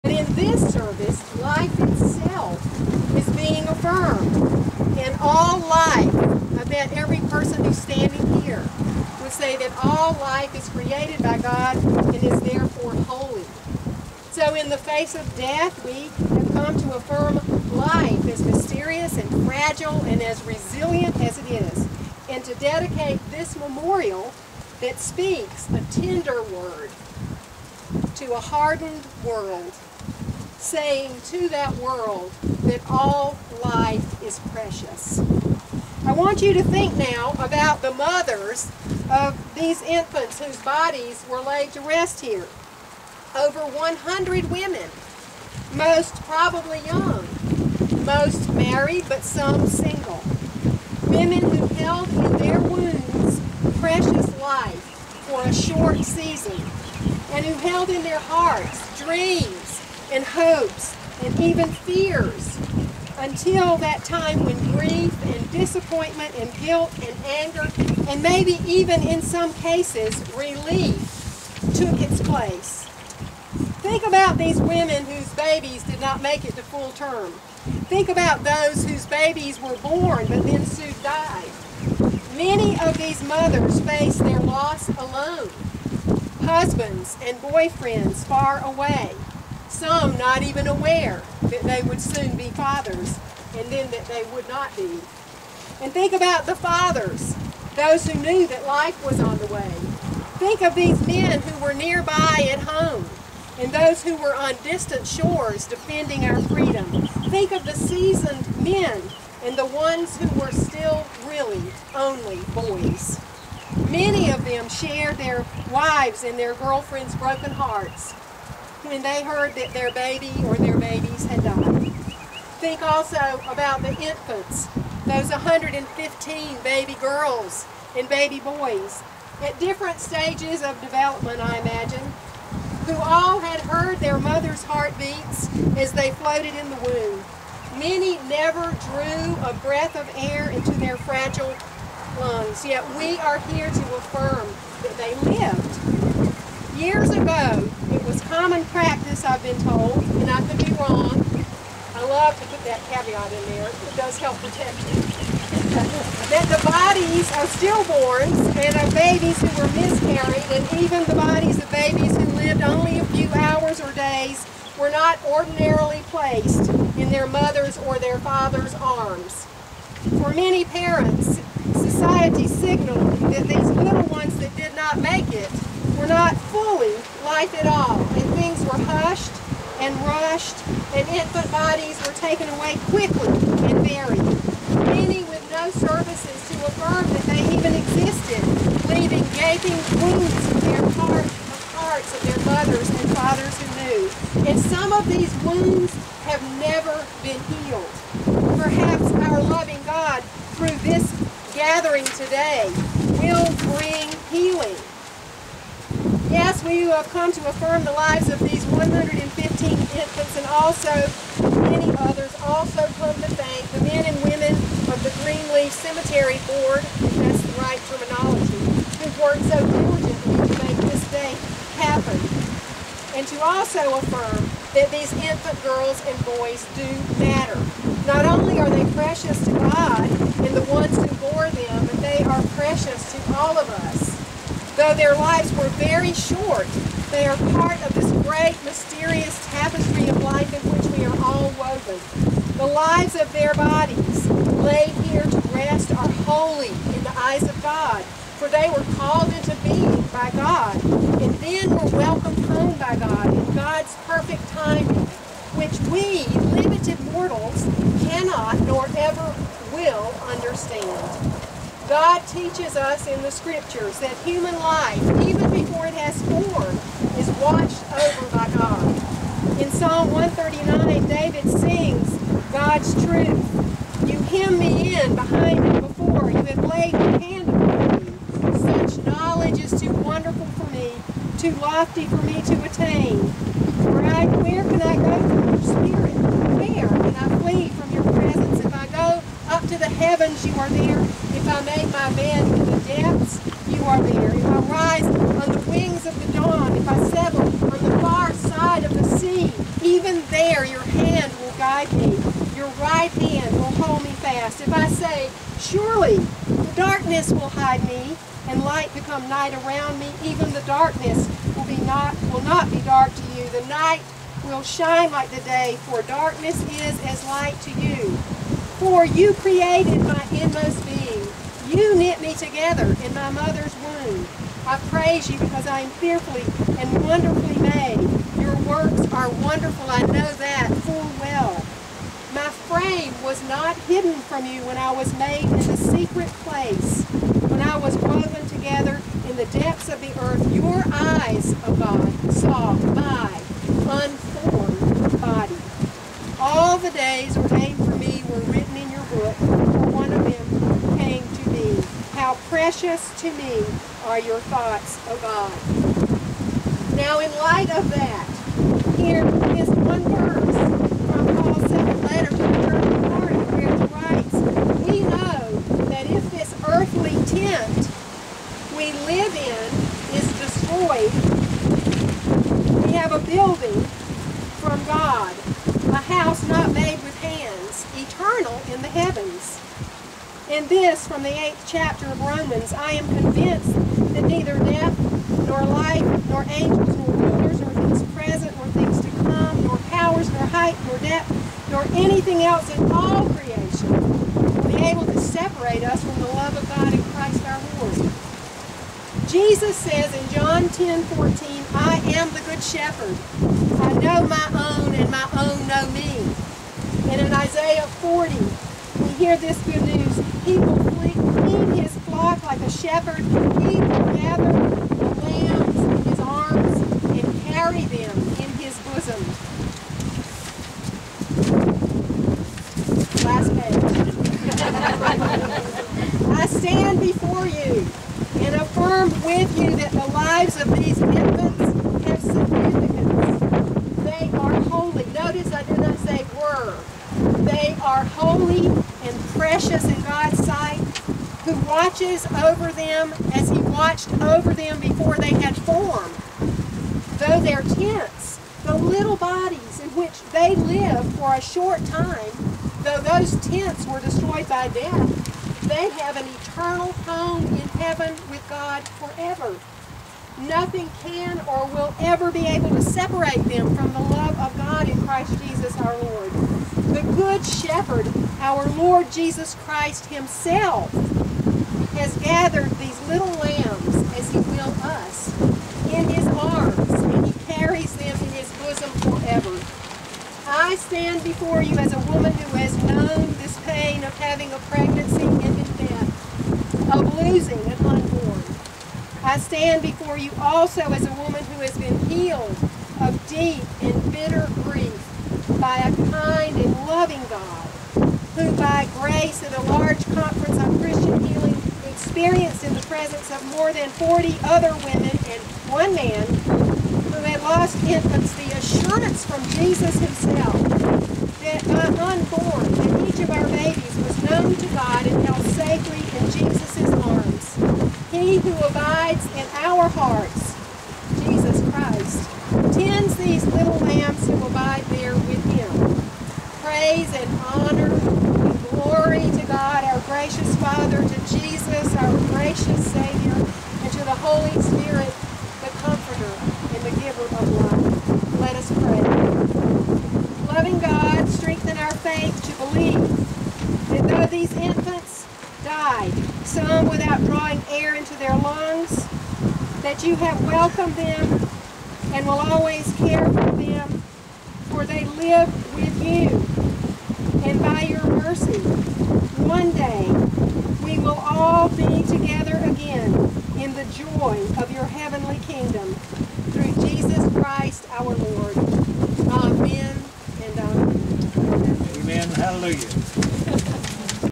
WWII Infants Memorial Message.m2ts
Reverend Mary D'Alessio presents a message that gives hope to the families of WWII infants buried in unmarked graves at Greenleaf Cemetery in Brownwood, TX, that affirms the importance of their short lives, explains the pain these families faced (especially during wartime) and gives hope to families that one day they will be reunited in Heaven.